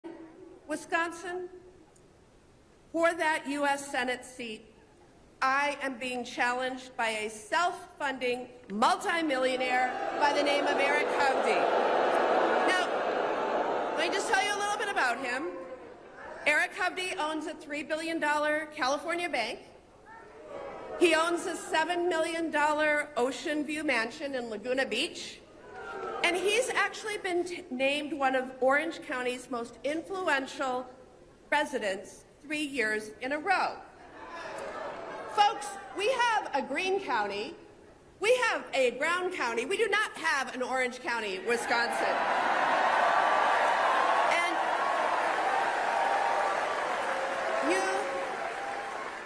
Eric Hovde is loved in Orange County California but in Wisconsin...
Sen. Tammy Baldwin (D-WI) slams opponent Eric Hovde for his ties to California: "Folks, we have a Green County. We have a Brown County. We do not have an Orange County in Wisconsin."